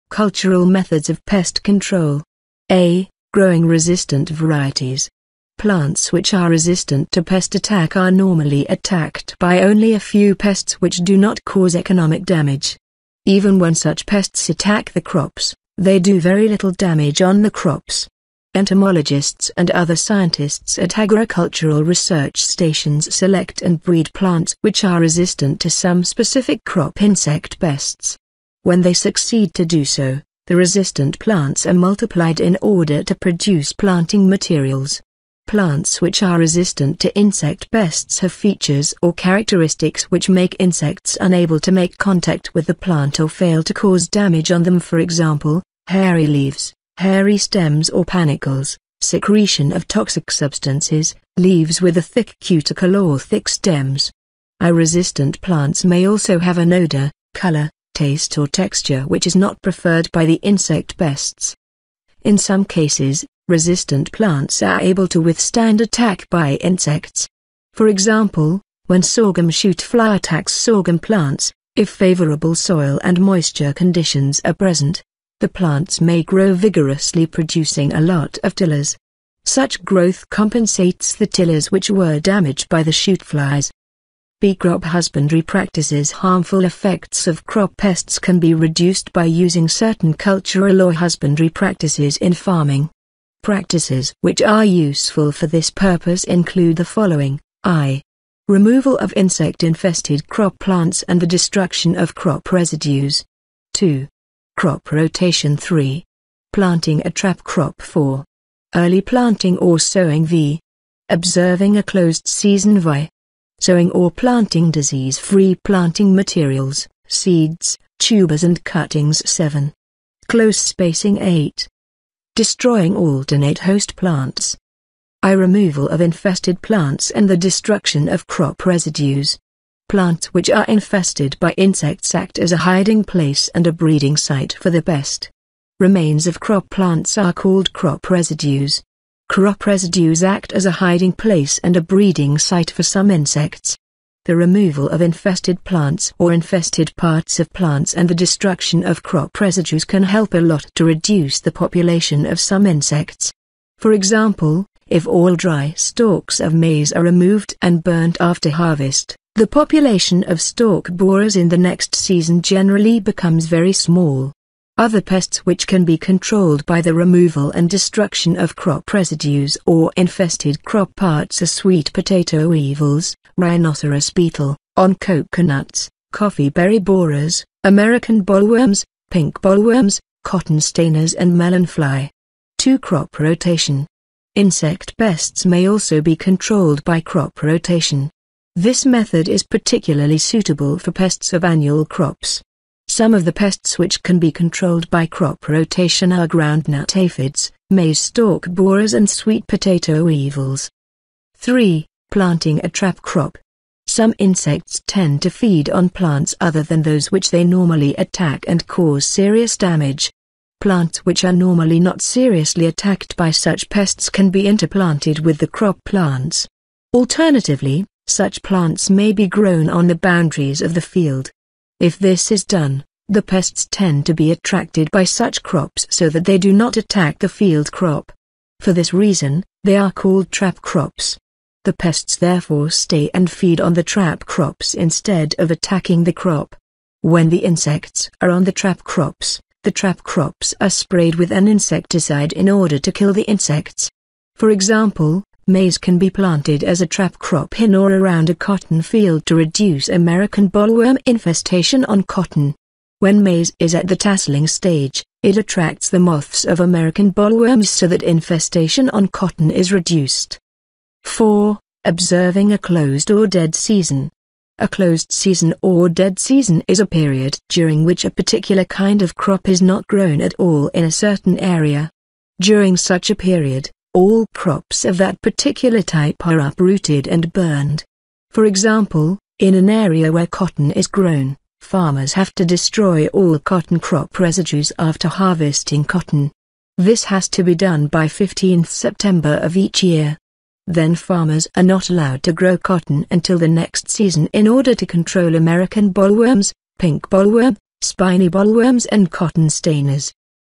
Cultural Methods of crop pest control part 1
Cultural Methods of pest control. Includes activites like; Growing resistant varieties and Crop husbandry practices
Harmful effects of crop pests can be reduced by using certain cultural or husbandry practices in farming. Practices which are useful for this purpose include the following:
i. Removal of insect-infested crop plants and the destruction of crop residues.
ii. Crop rotation
iii. Planting a trap crop
iv. Early planting or sowing
v. Observing a closed season
vi. Sowing or planting disease-free planting materials (seeds, tubers and cuttings
vii. Close spacing
viii. Destroying alternate host plants.